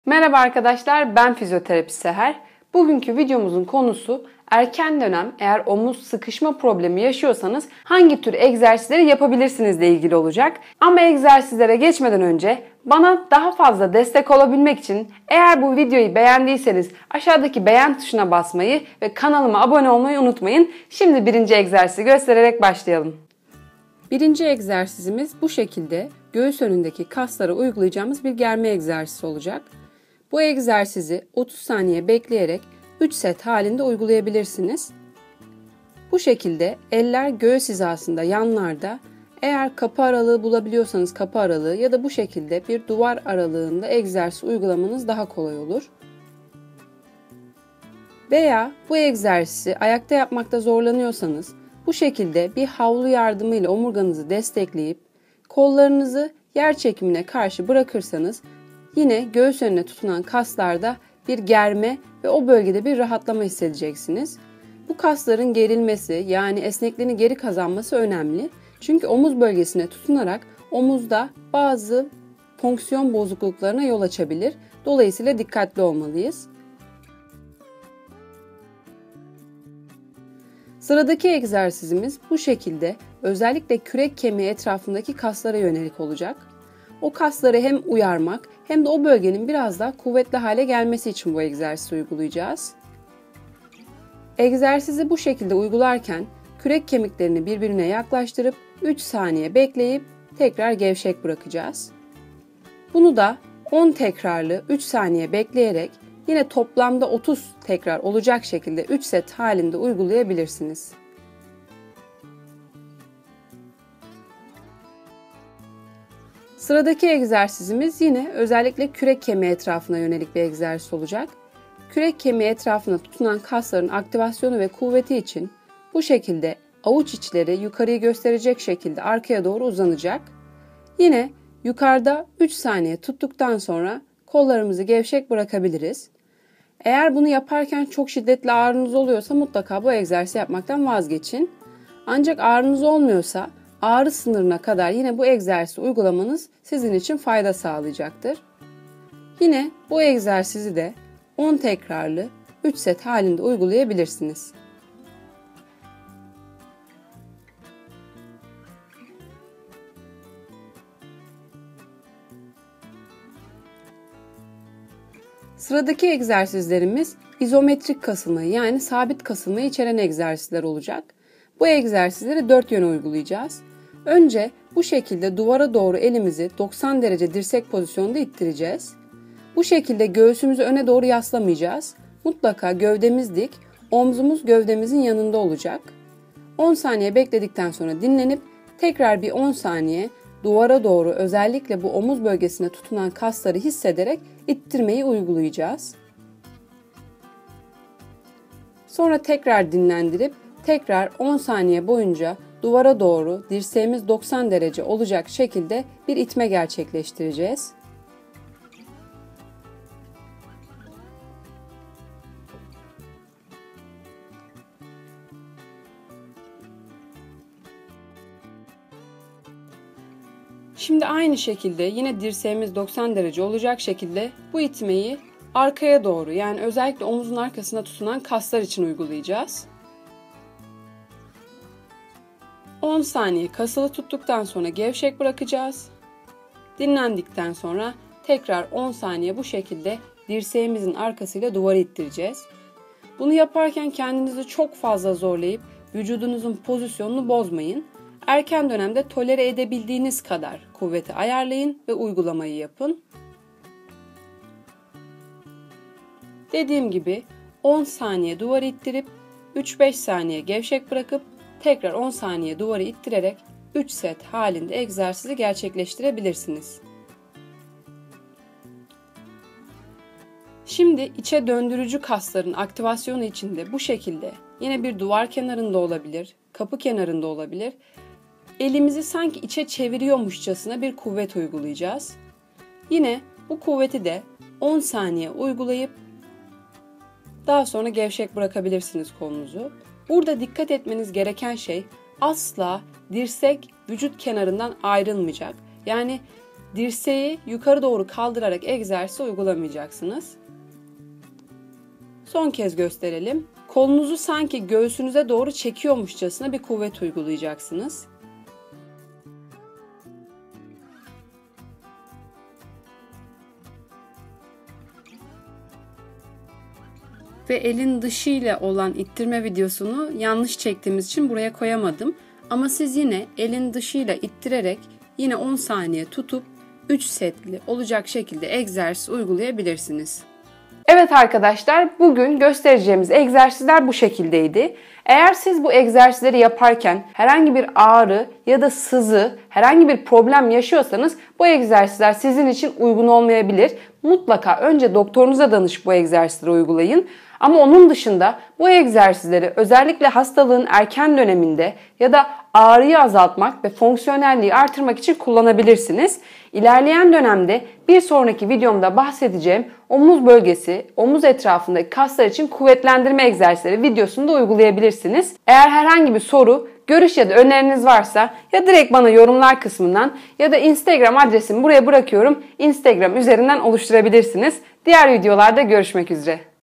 OMUZ AĞRISI İÇİN EGZERSİZLER (SIKIŞMA SENDROMU)
Merhaba arkadaşlar, bu videomda size omuz ağrısında veya sıkışma sendromunun erken döneminde uygulanabilecek egzersizleri örneklemek istedim.

Bu videolar tamamen eğitim amaçlıdır. Doktorunuza danışmadan uygulamanız doğru değildir. Gerçek tanı ve tedavi doktor ve fizyoterapistiniz aracılığıyla gerçekleştirilmelidir.